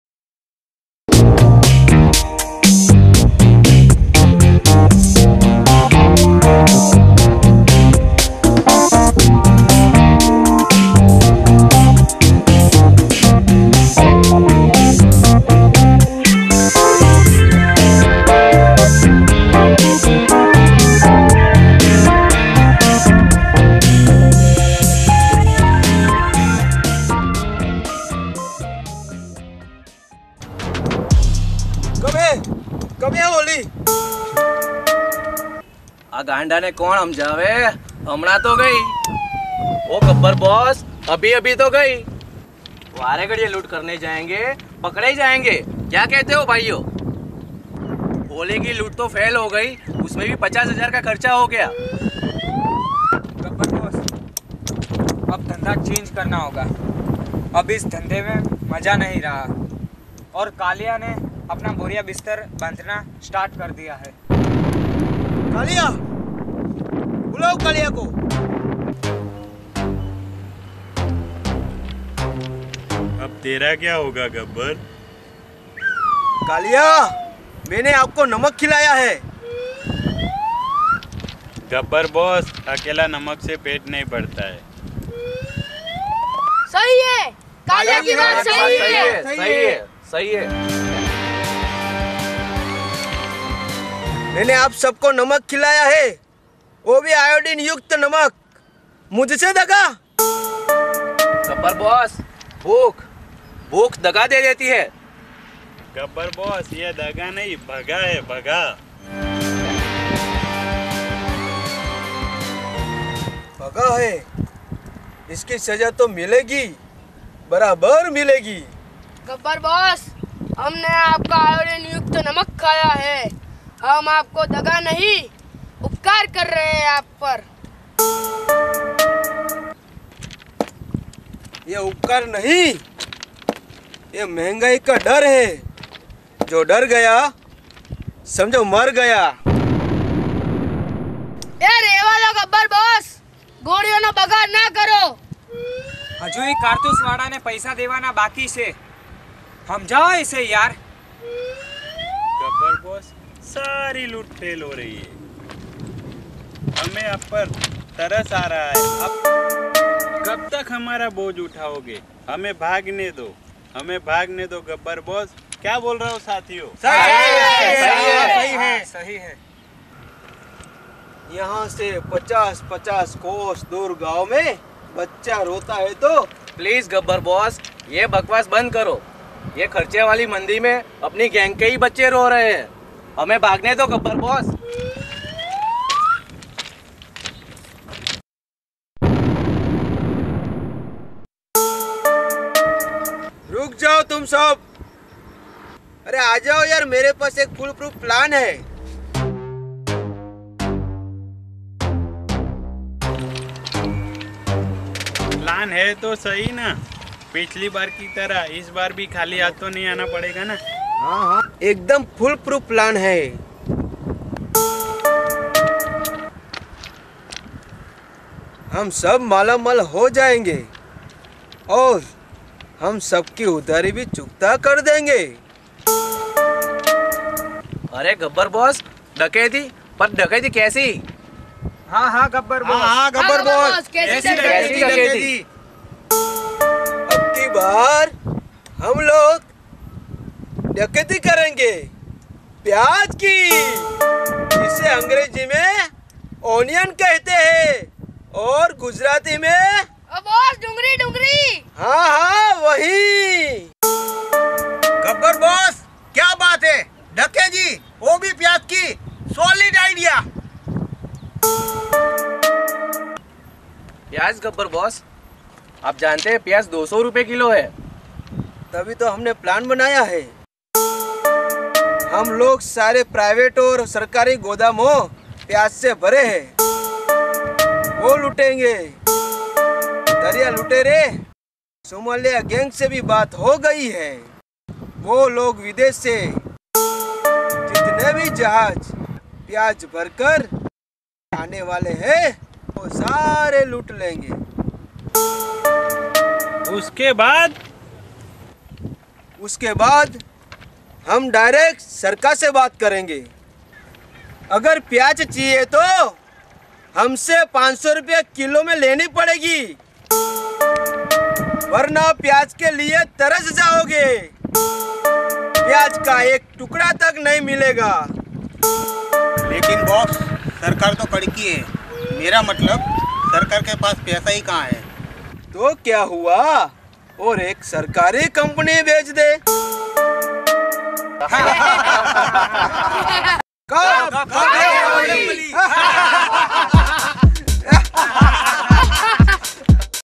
GABBAR KI DAKAITI PYAAZ
DUE TO INCREASE IN ONION PRICE, GABBAR DECIDED " AB KI BAAR DAKAITI PYAAZ KI "  IT IS SIMPLE COMEDY SHORT FILM.  HOPE AUDIANCE MAY LIKE IT.